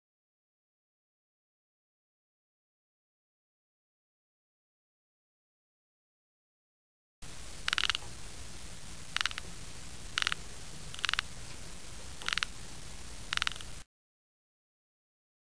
Weird bug
This is a strange bug we found on our trampoline last summer... have you ever seen anything like it!?